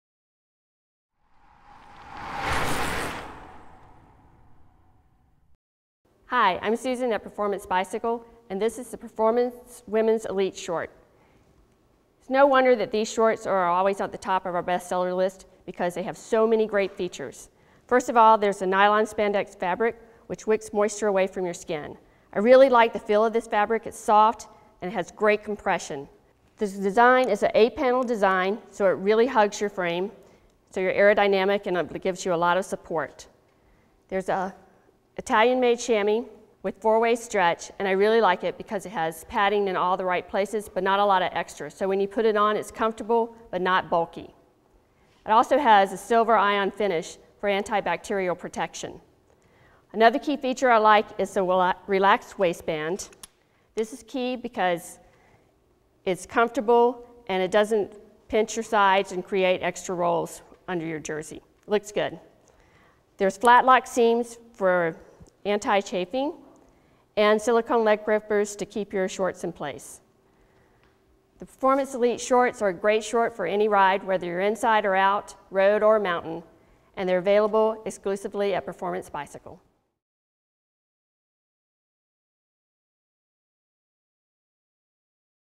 Women's Elite Cycling Short Review from Performance Bicycle
We combined the best technical properties with a soft feeling fabric, the comfort of a premium chamois, the chafe-free experience of silicone leg grippers and a relaxed waist, into our "go anywhere, do anything" women's cycling shorts.  The result is the Women's Elite Short Sleeve Jersey from Performance bike.  Click on the link for full product details.